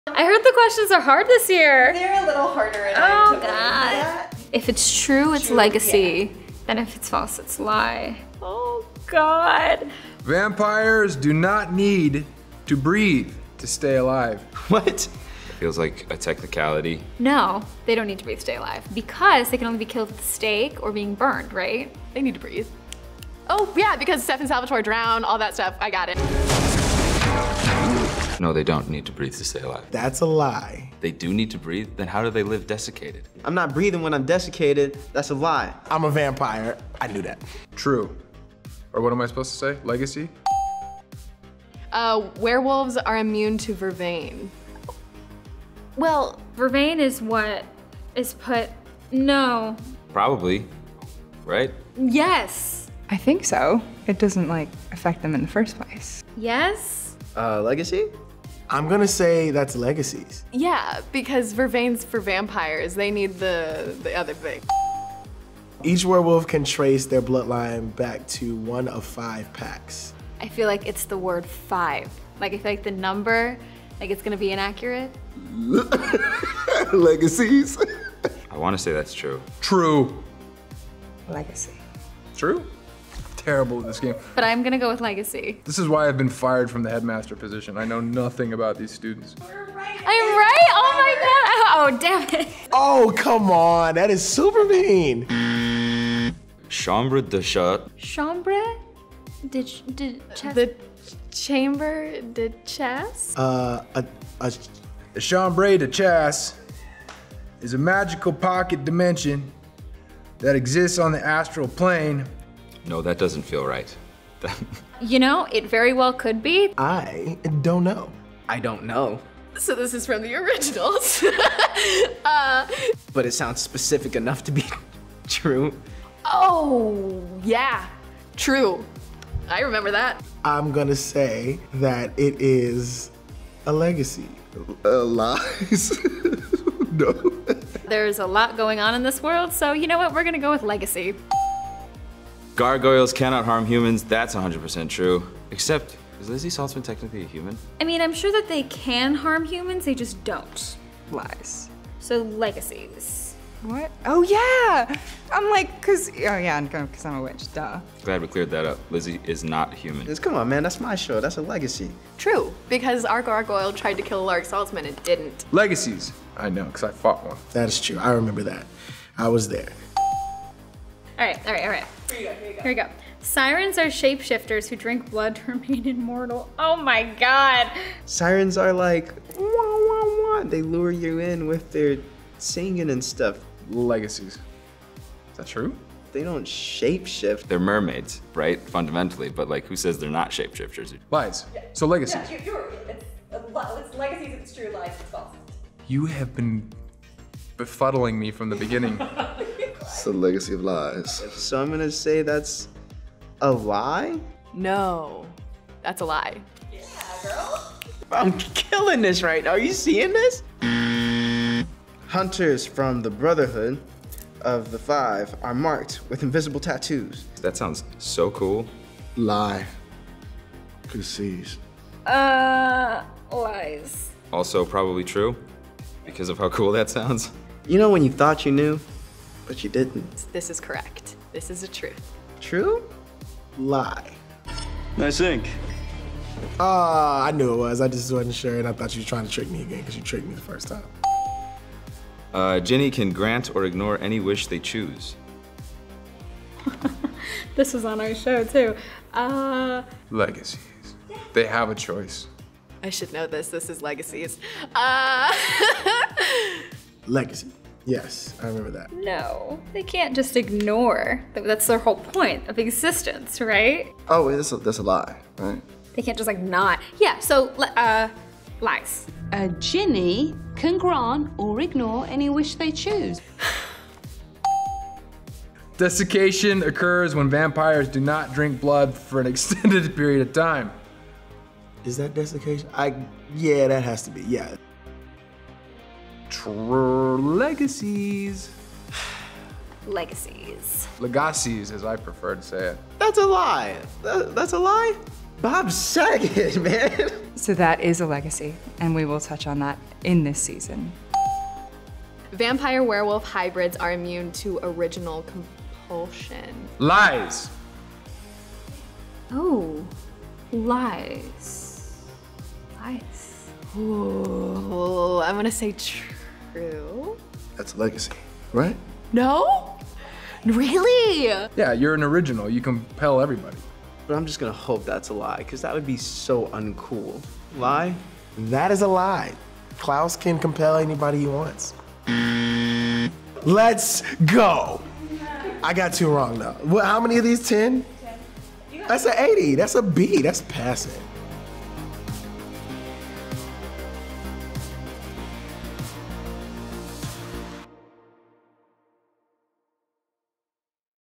Legacies Cast Plays The Vampire Diaries Trivia Game: Part 2
We quizzed the cast of Legacies, including Matthew Davis (Alaric), Aria Shahghasemi (Landon), Danielle Rose Russell (Hope), Kaylee Bryant (Josie), Quincy Fouse (MG), Chris Lee (Kaleb), Jenny Boyd (Lizzie) and Peyton Alex Smith (Rafael), on their Vampire Diaries and Originals knowledge. Watch out video to see how they did!